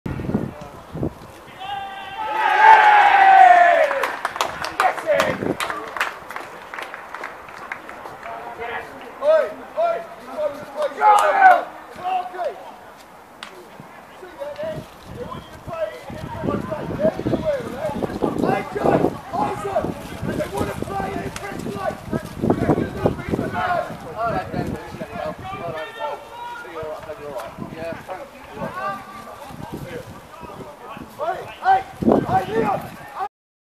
Ilford 4-2 Eton Manor. Essex Senior League. Wed19Oct2016
Despite letting an early lead slip, Ilford dramatically turn things around in the second half at Cricklefield, to keep pace with the leading pack